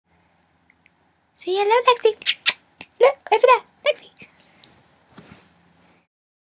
Introducing Lexi!
Just brought home our 10 month old puppy, Lexi! She's a Cavalier King Charles Spaniel, and she's a princess. (Had to upload to youtube as Facebook's blocked me uploading videos, boo.)